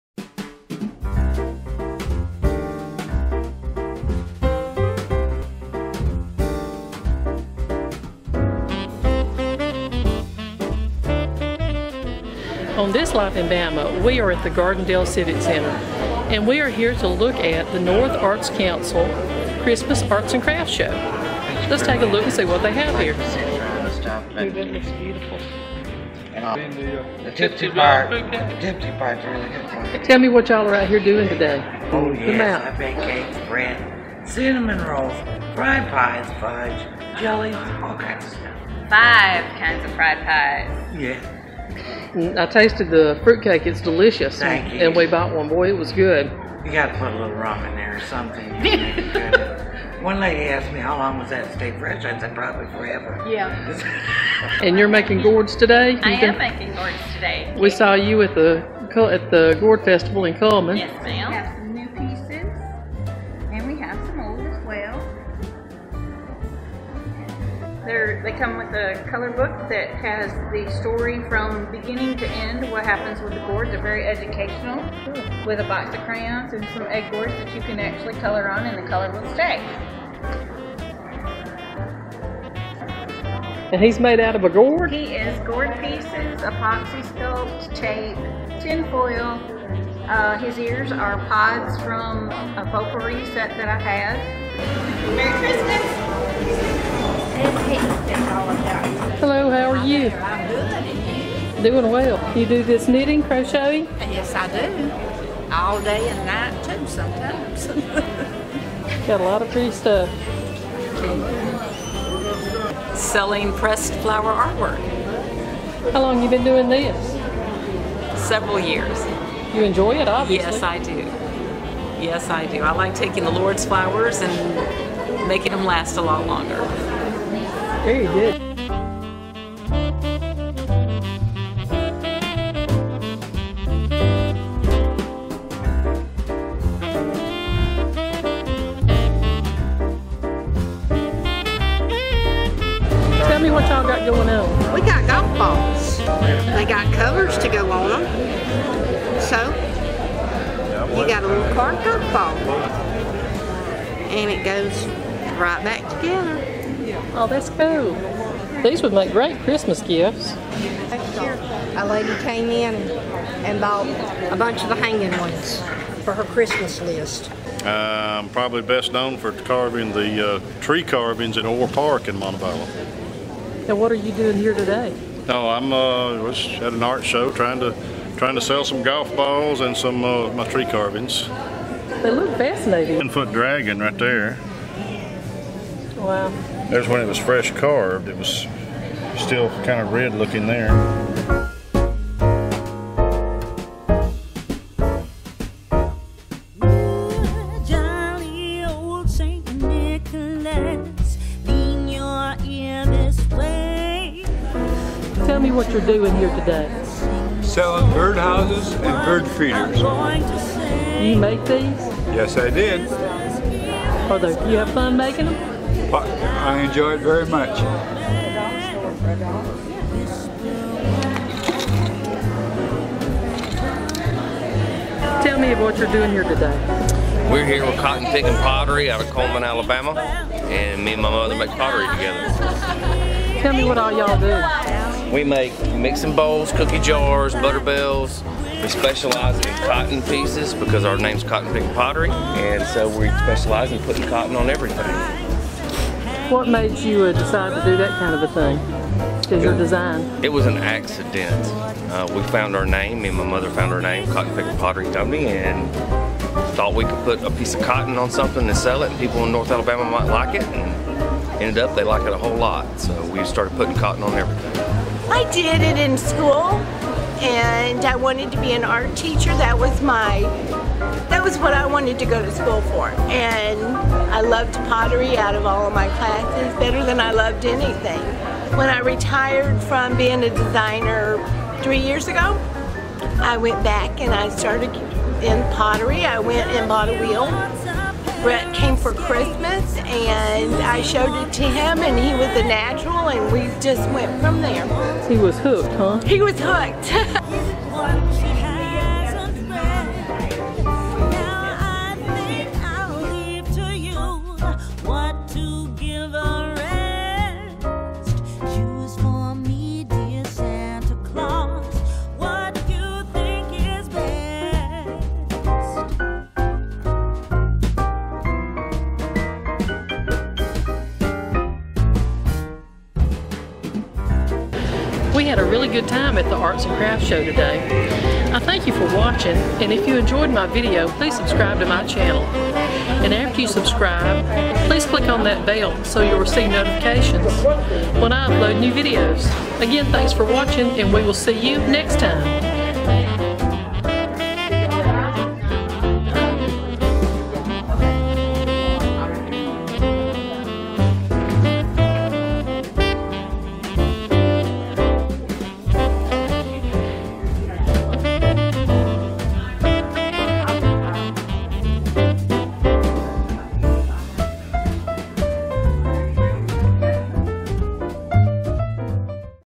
Christmas Arts & Crafts | Gardendale | Life In Bama
Join us as we talk with some of the folks at the Annual North Arts Council Christmas Arts & Crafts show at the Gardendale Civic Center. Everything from food to Christmas folly is offered here! We have a great time meeting everyone and seeing their arts & crafts.
This is part 1 of two parts so make sure you see the 2nd half.